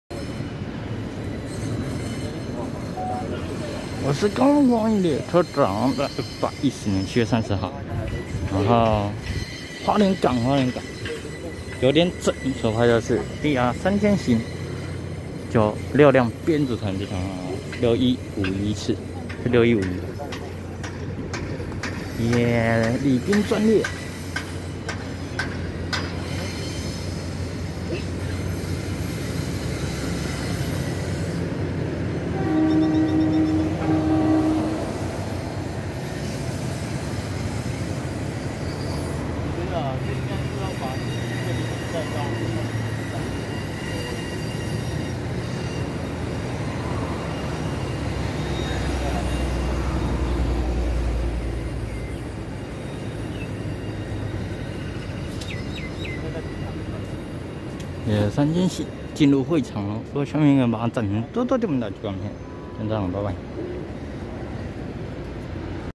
6151次開往花蓮八號碼頭DR3000型試運轉DR3001+DR3071+DR3002+DR3003+DR3072+DR3004禮賓專列六輛編組柴聯自強號，和EMU3000型皮蛋豆腐號日本母船
110/7/30花蓮港華東橋9:00分拍攝6151次花蓮發車經由花蓮臨港線開往花蓮八號碼頭DR3000型試運轉DR3001+DR3071+DR3002+DR3003+DR3072+DR3004禮賓專列六輛編組柴聯自強號，和EMU3000型皮蛋豆腐號日本母船